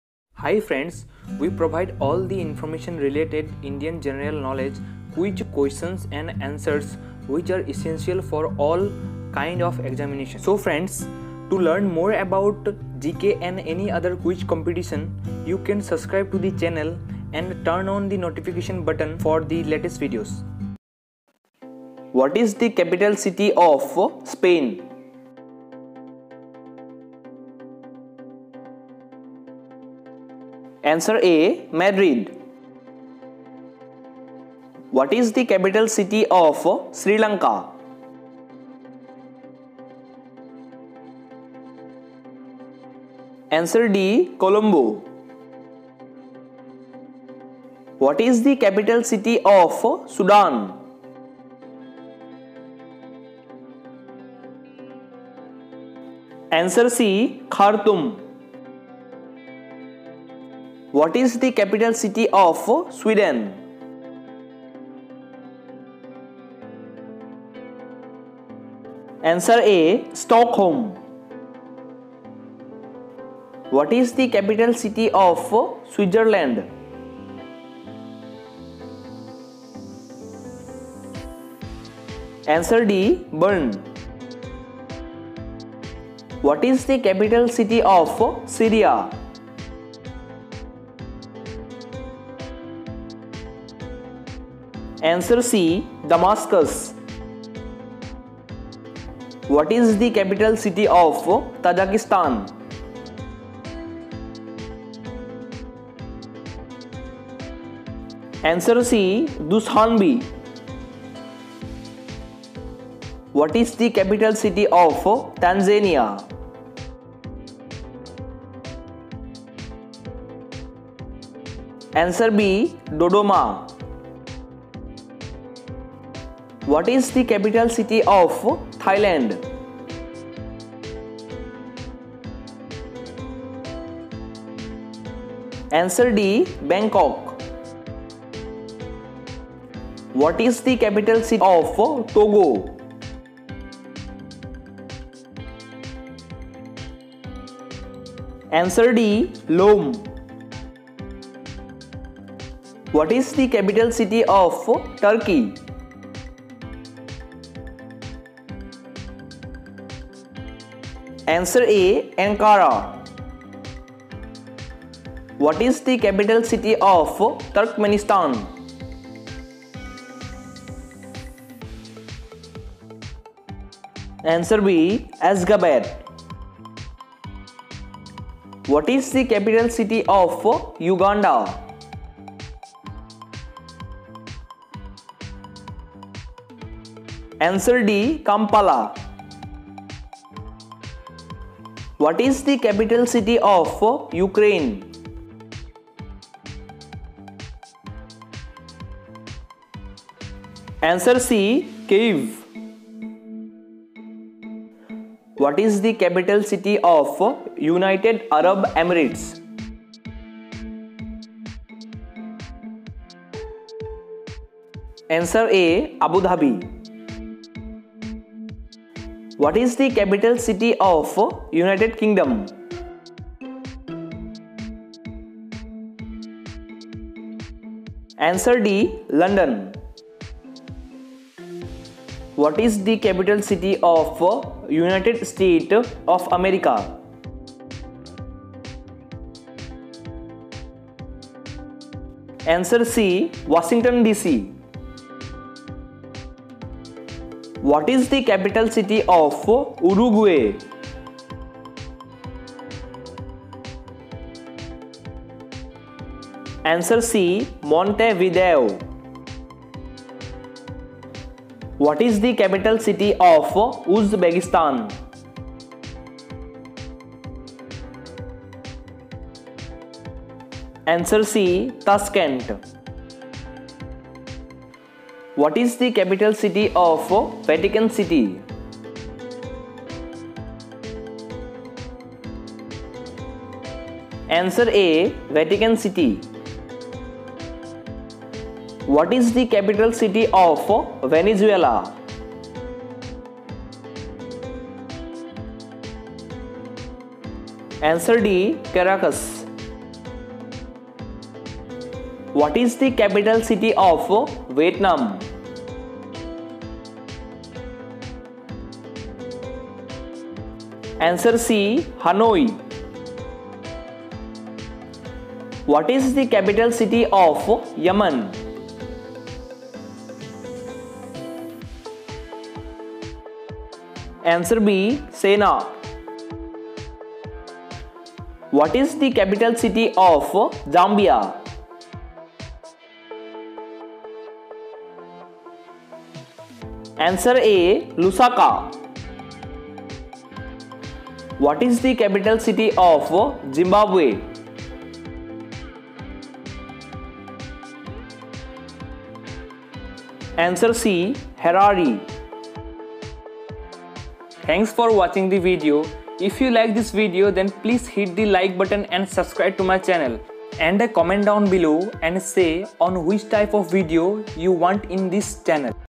25 question & answer about World capitals | World Countries & Capitals Quiz | GK quiz 2022 | part 7
25 Basic GK Questions and Answers of countries and capitals quiz 2022 | World GK Quiz about it's capitals in English | GK Question and Answer || Part 7

In any spelling mistakes in the above video than pls forgive me while creating the it take too much time if any mistake done than pls sorry

In this video, 25 important questions on worlds Countries and It's Capitals GK quiz Questions and Answers in English given below.